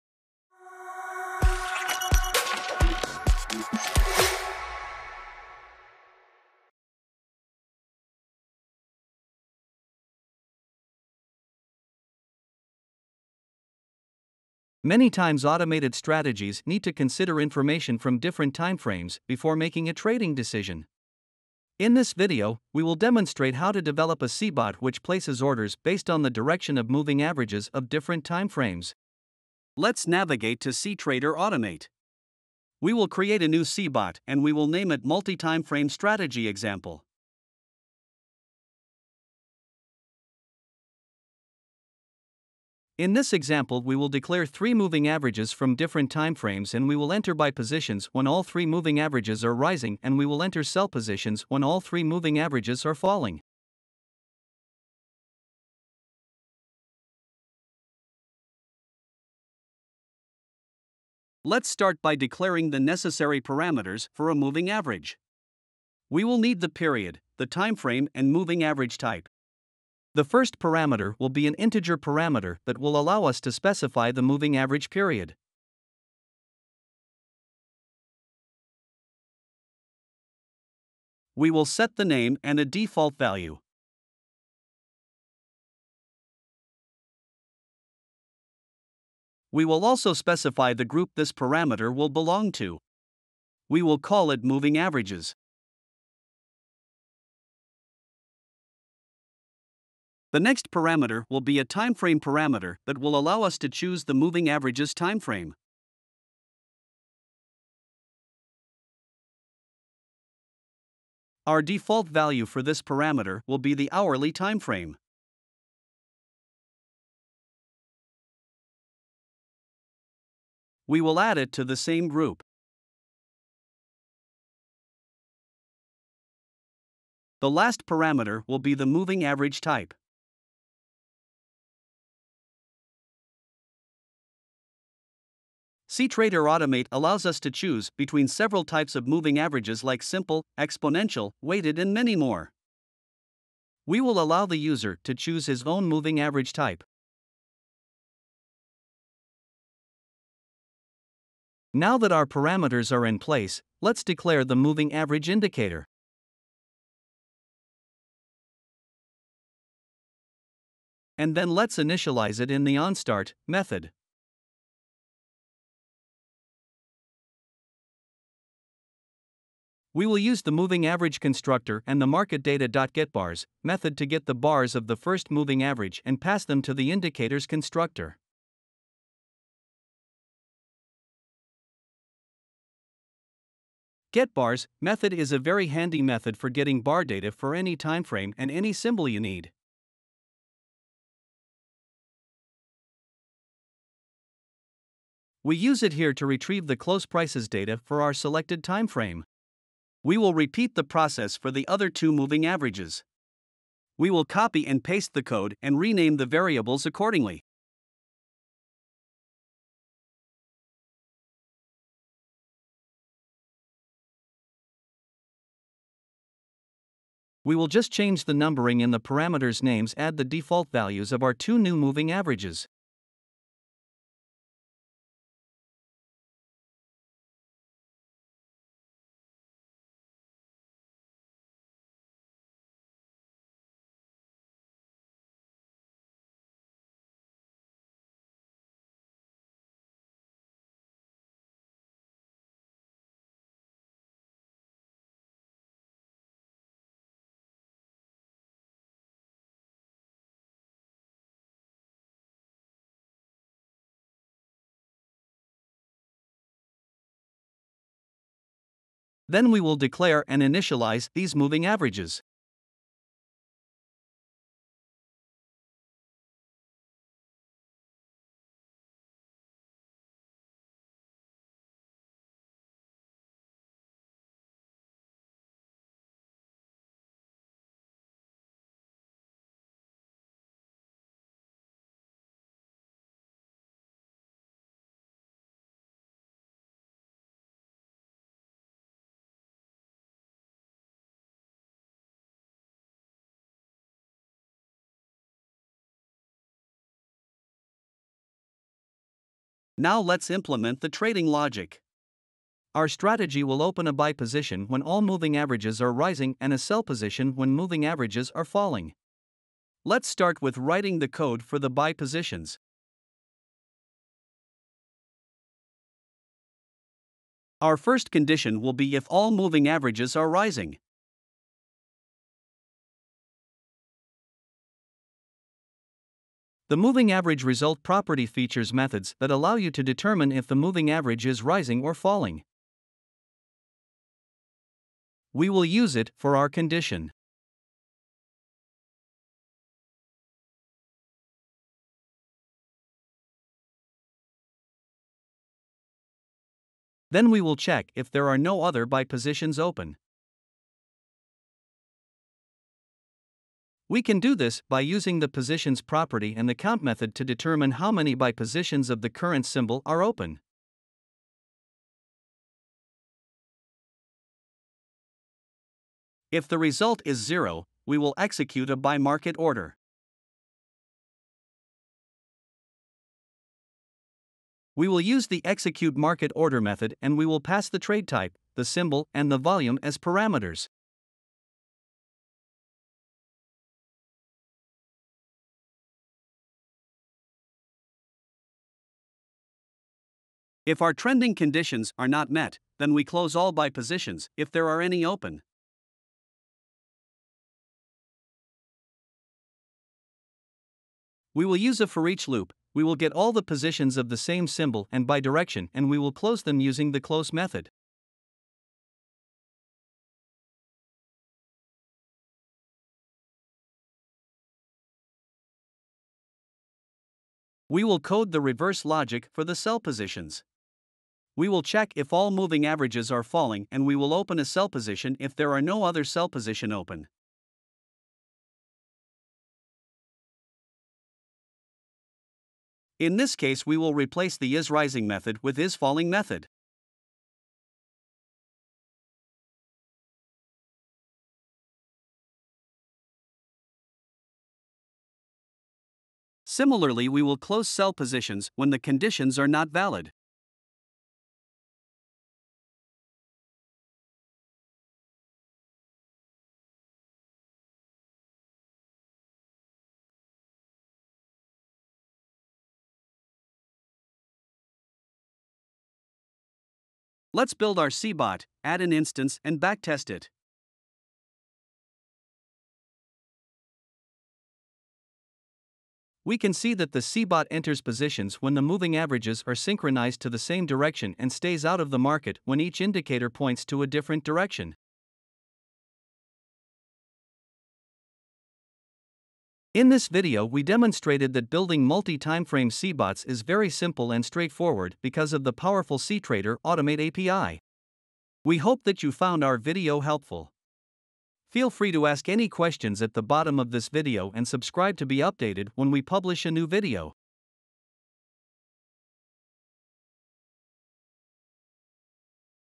Coding Multitimeframe Strategies in cTrader
This video demonstrates how you can easily code cBots that trade multiple timeframes at the same time.

Chapters:
00:09 Introduction
00:58 Declaring cBot parameters
04:56 Implementing the trading logic
07:43 Backtesting the multiframe strategy